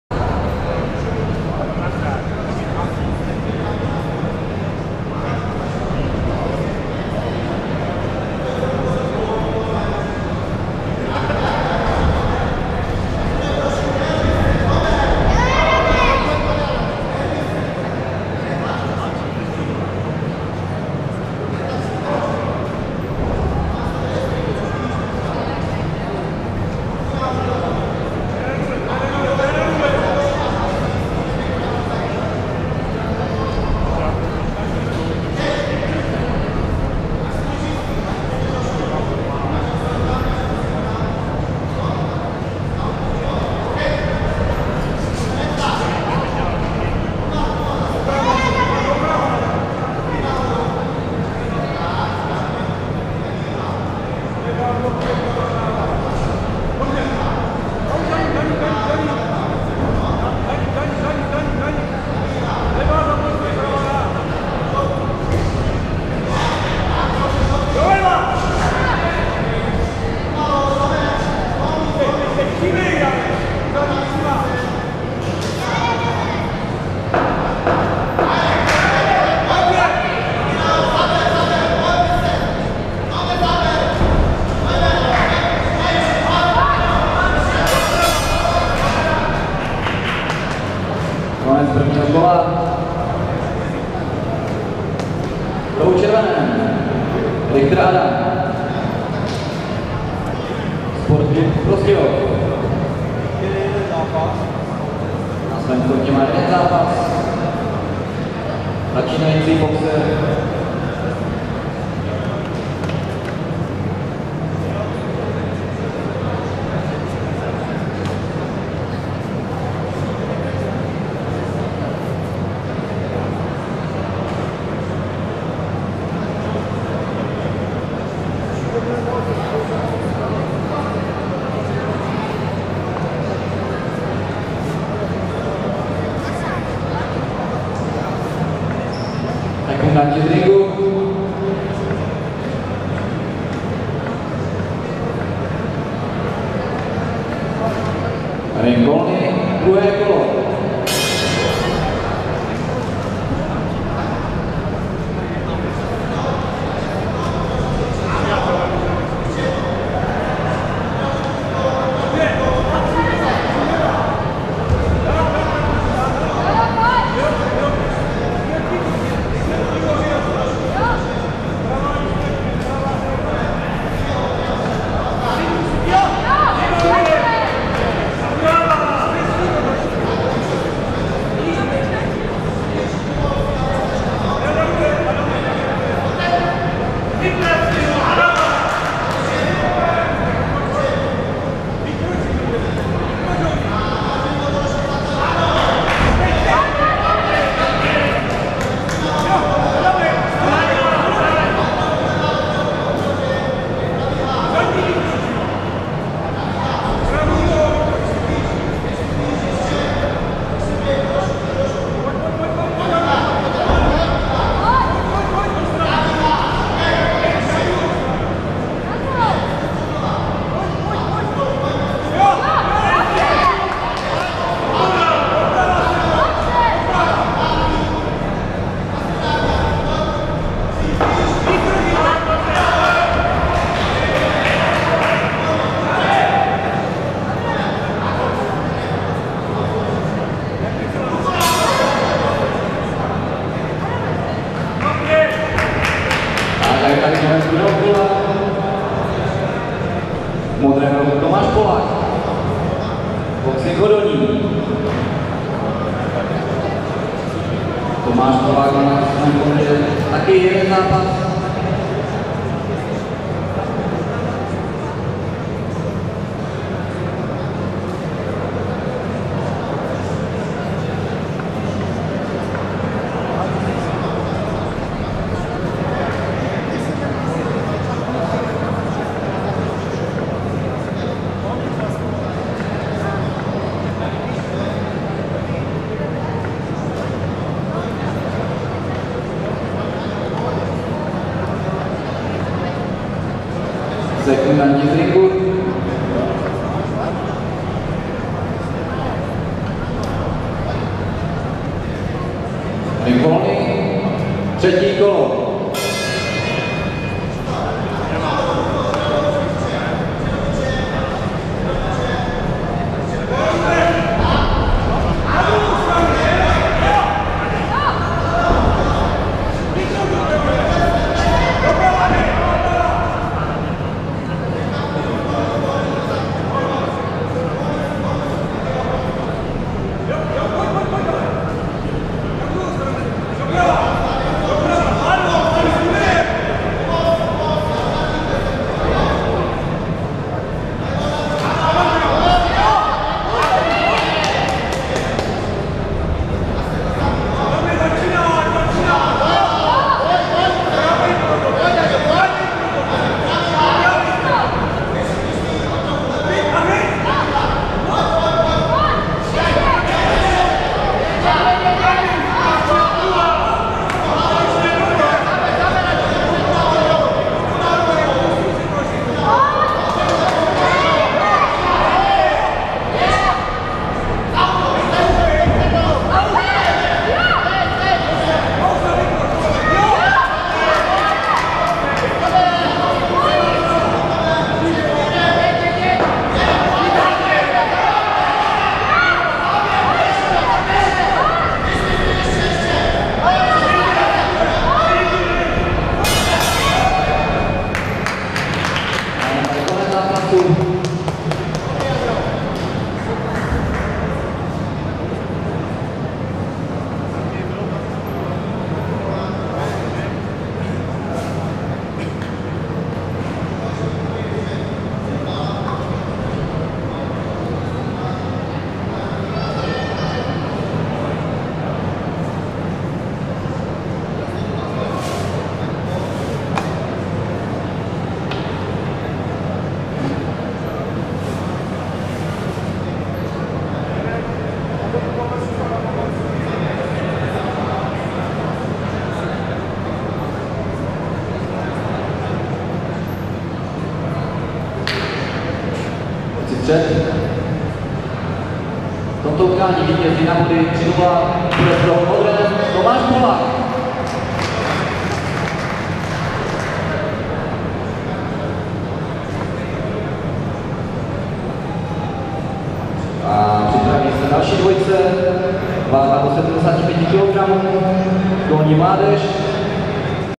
Richter Adam proti Polák T.
Richter Adam Sport klub Prostějov v červeném rohu proti Polák T. v modrém rohu z BC Hodonín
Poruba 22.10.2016